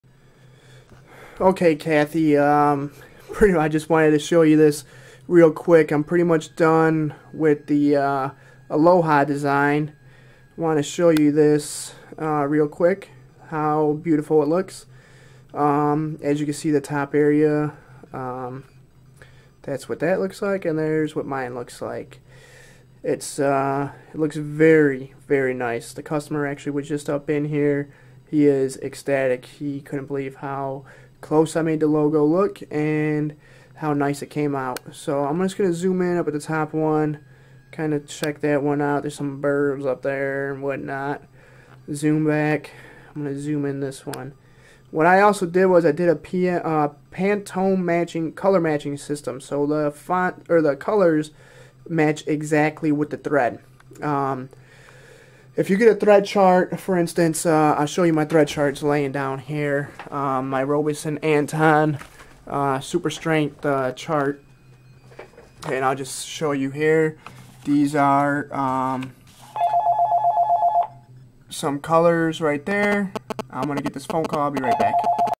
Embroidery Digitize Finished Logo using Wilcom ES 2006 by Tim Ruffner
Embroidery Digitize Finished Logo using Wilcom ES 2006 by Tim Ruffner . I show you a finished product after digitized. Follow my other videos to learn how to digitize more embroider and chenille patches using Wilcom ES 2006.